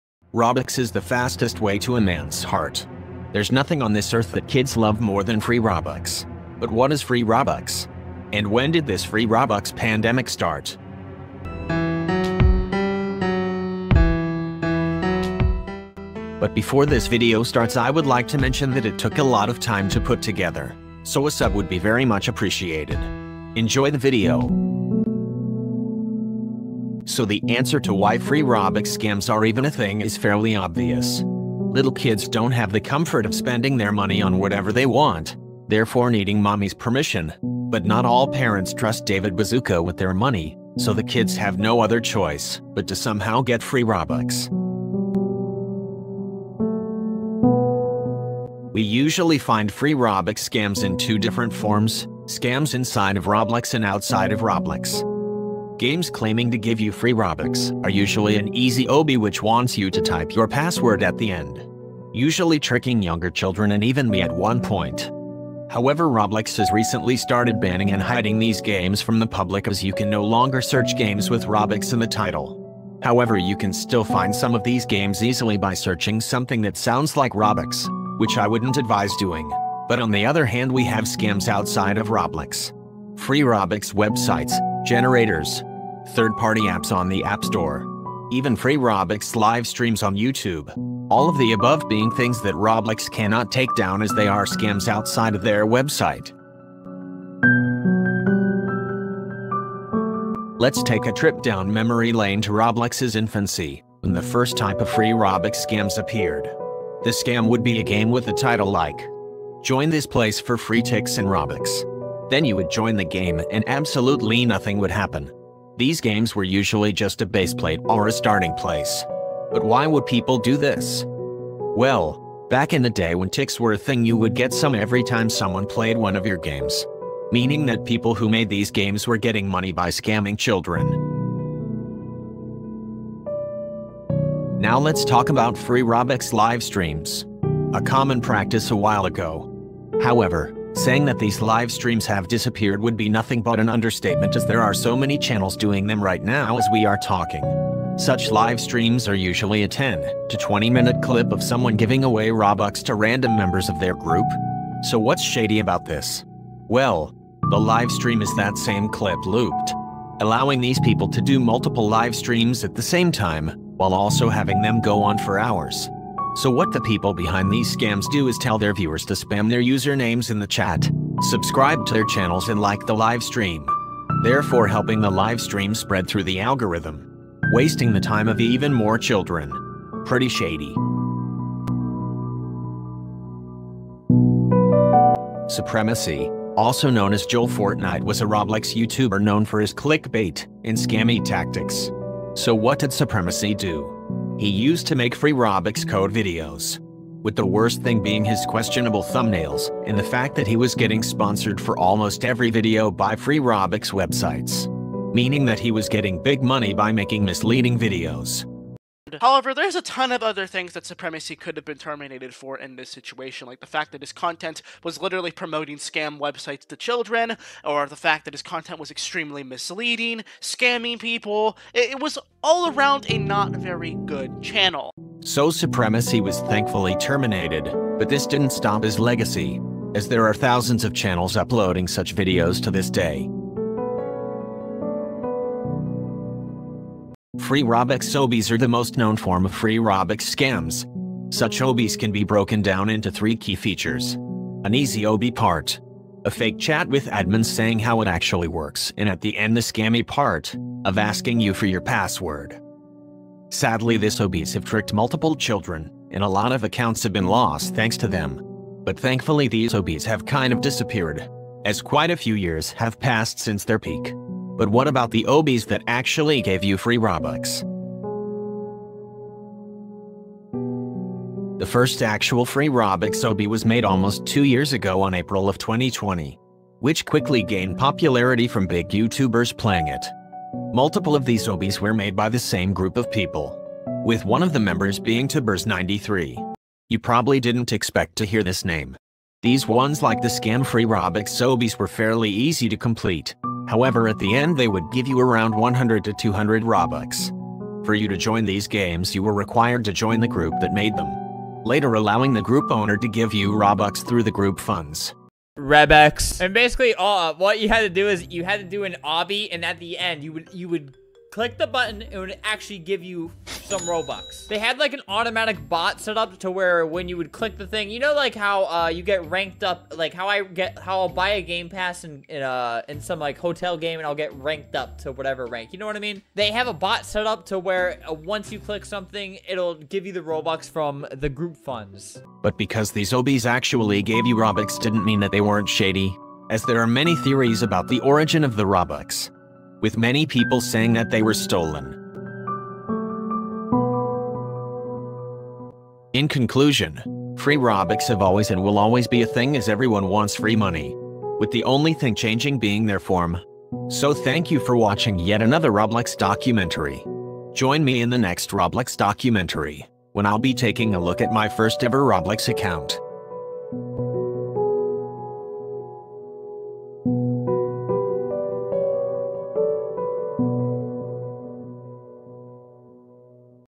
Free Robux: The History (Roblox Documentary)
Today I talked about free robux and its evolution over the years. Can you actually get free robux?